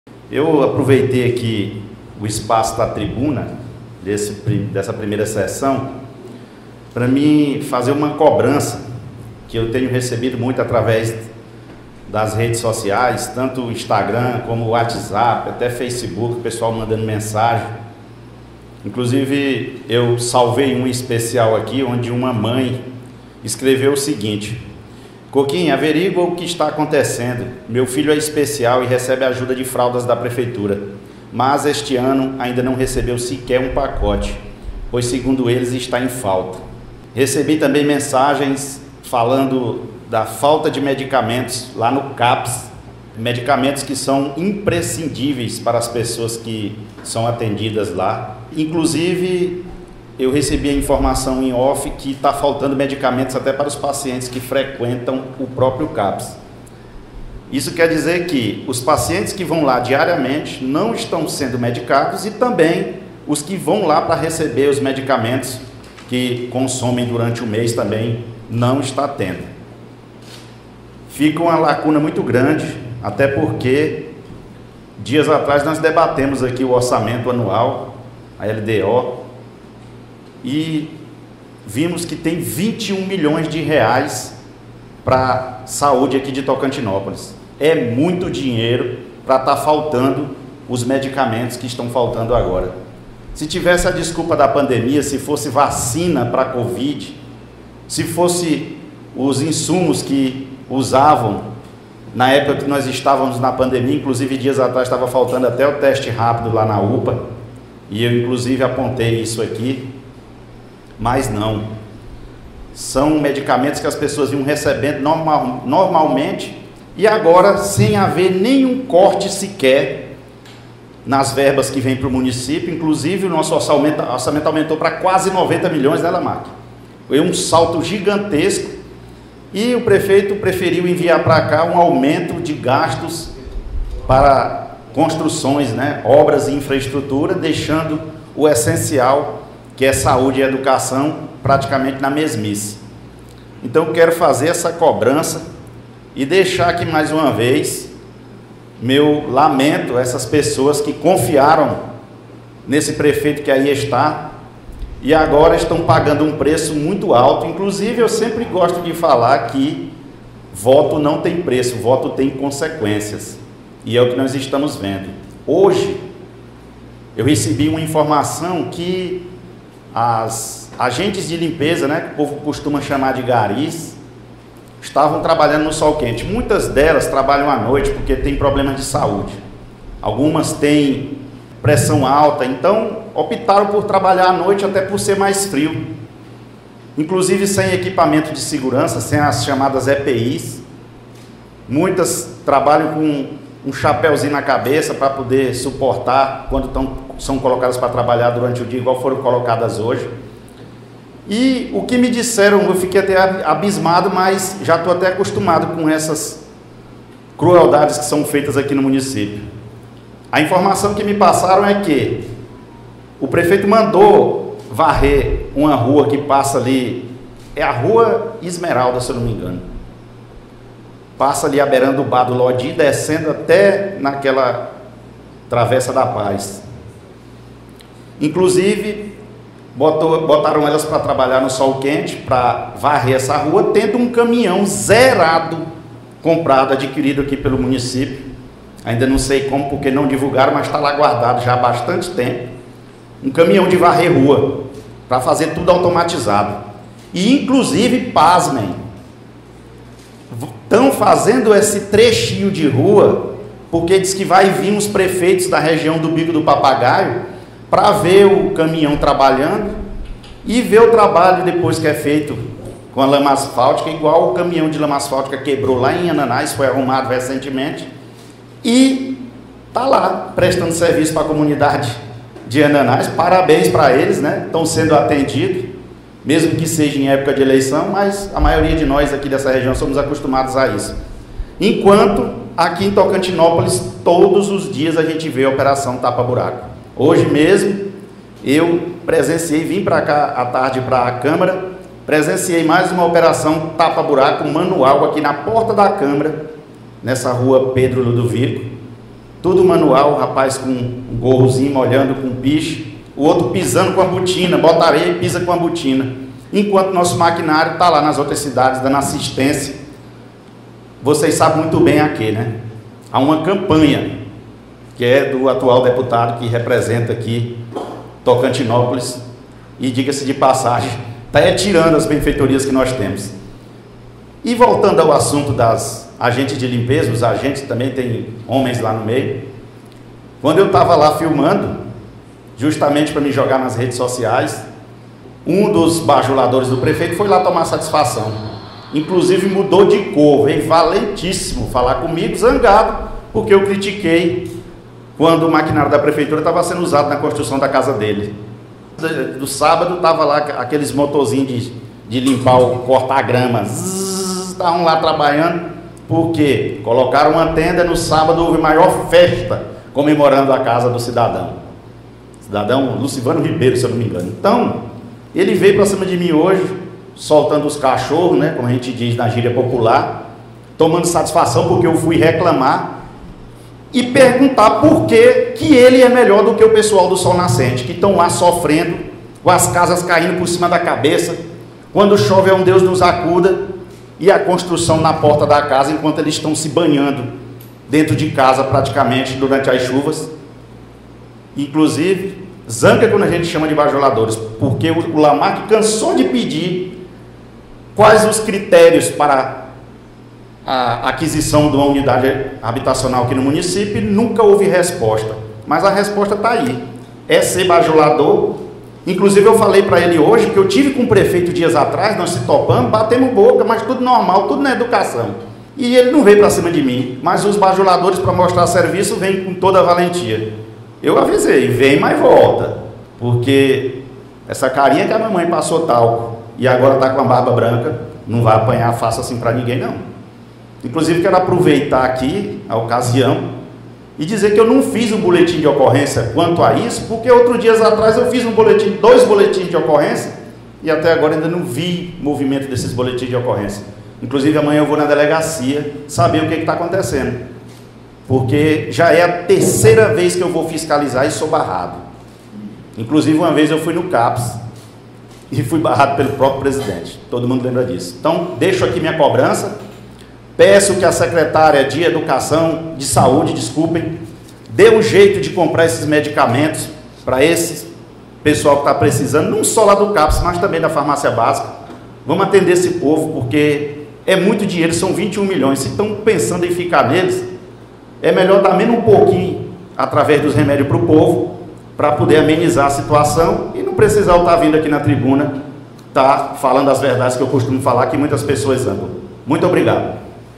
Na Tribuna, Vereador Roberlan Cokim reclama da falta de medicamentos no CAPS
Usando de seu direito a fala na tribuna da câmara, o vereador Roberlan Cokim (PSC-TO), utilizou-se do espaço para reclamar da falta de medicamentos no CAPS (Centro de Atenção Psicossocial), além de fraudas que eram doadas às mães com crianças especiais. O camarista falou ainda de um caminhão de varrer ruas que estava guardado na garagem da prefeitura enquanto as agentes de limpeza eram obrigados a trabalhar no sol  quente sem qualquer equipamento de proteção individual.